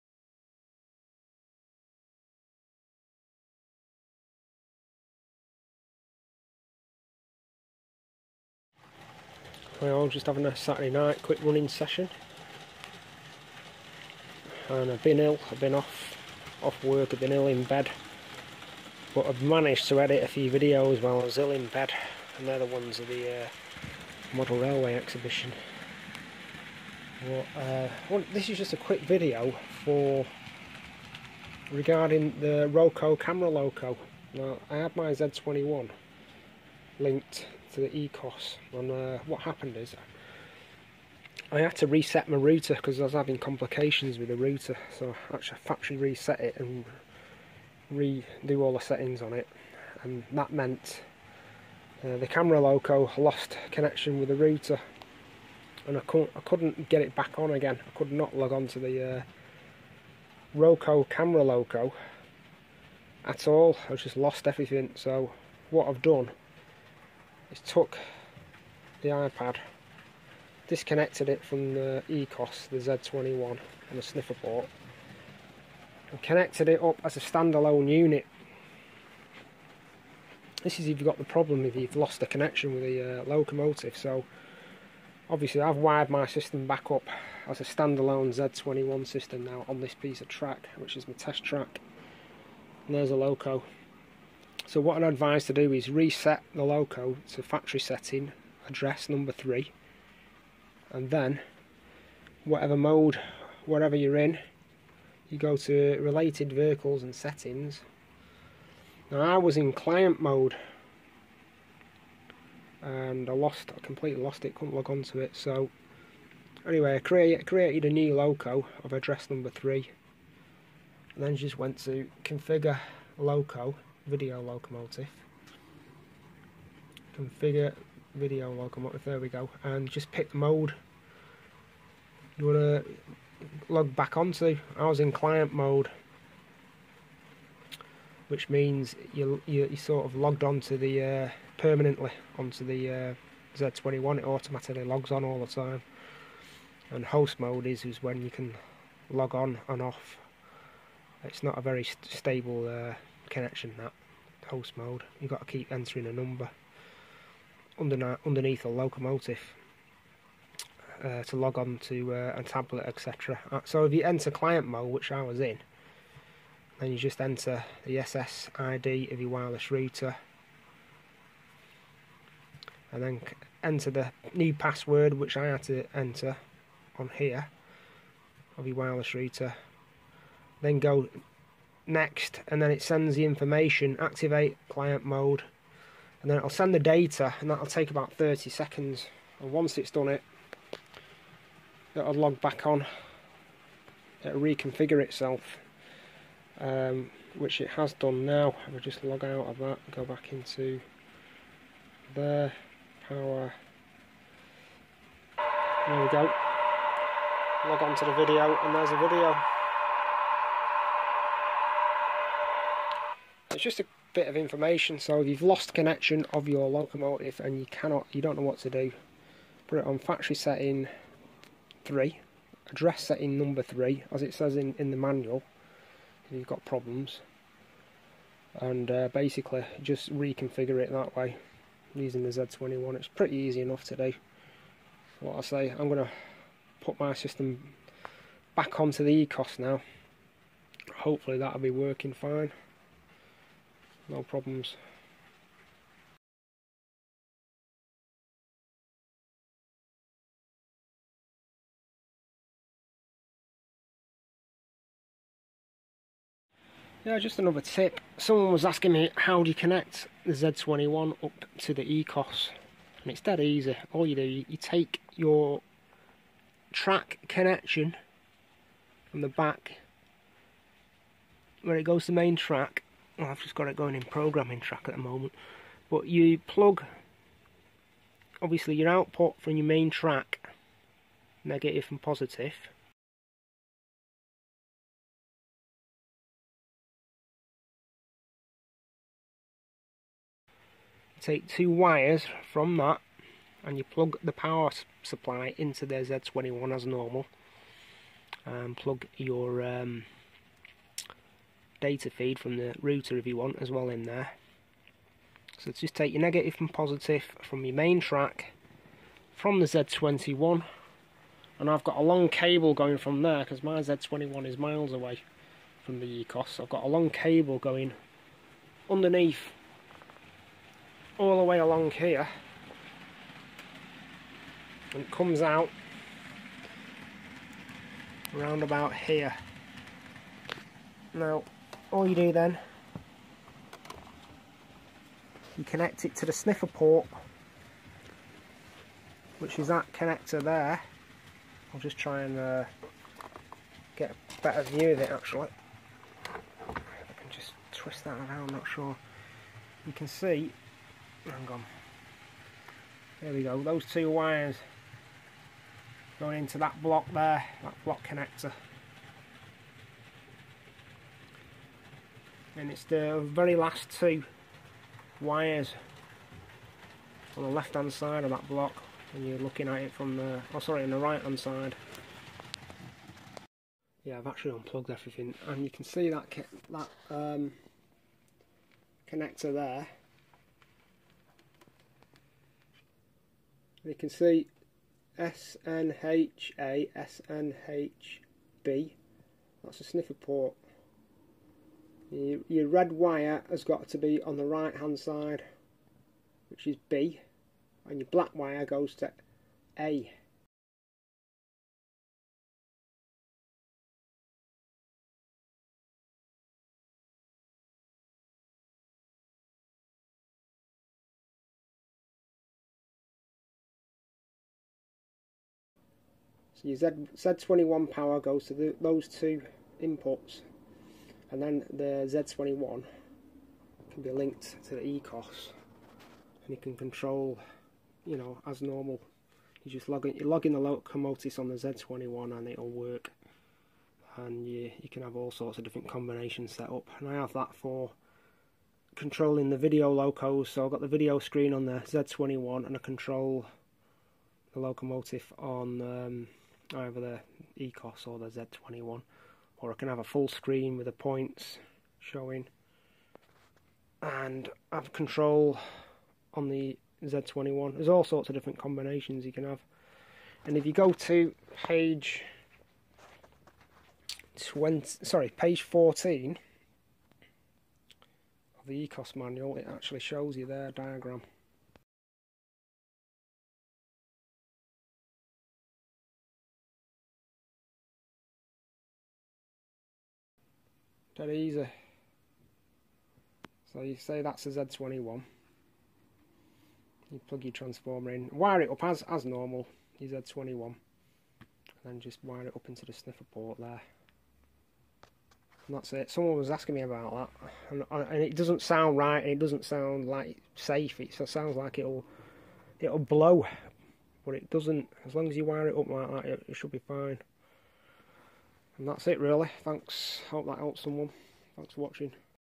Resetting Roco camera loco , Z21 sniffer port connection to Ecos.
To reconfigure the roco camera loco out of client mode set to address 3 factory default and reset the loco on the z21 app using the z21 hardware on the main line feed.

NOTE: if you have changed the name - SSID of your router then enter the correct name or the default name of the router on the z21 app when promted otherwise it will not enter client mode again.

Before you start you may also need to delete the old address or default address of the camera loco completely from the z21 Z21 loco database so a lost client connection can be reestablished.